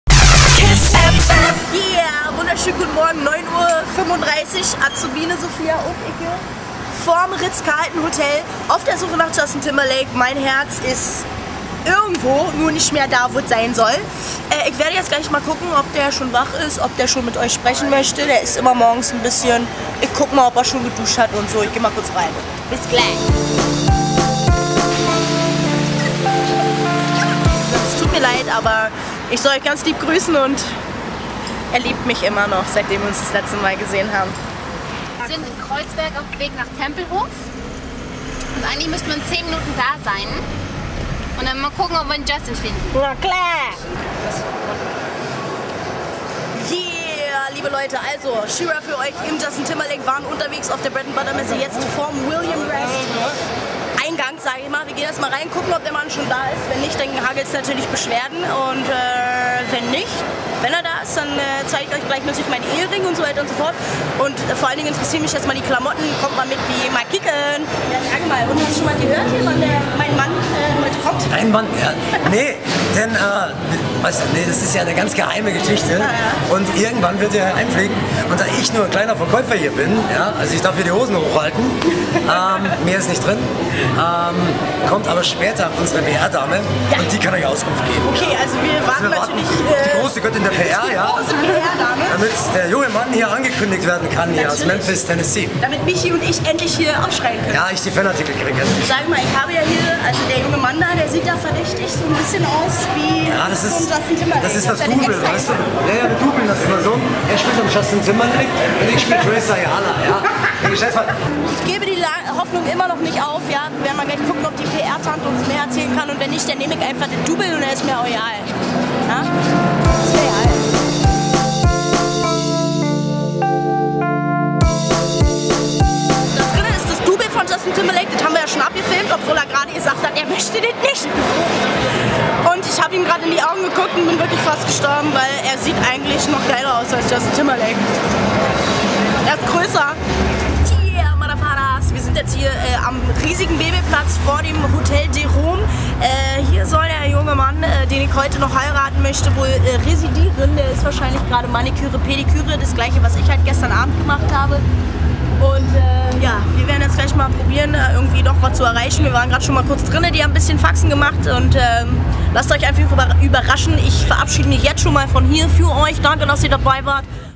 98.8 KISS FM: She-Raw und Azubiene Sophia suchen Justin Timberlake in der Muddastadt
98.8 KISS FM: She-Raw und Azubiene Sophia suchen Justin Timberlake in der Muddastadt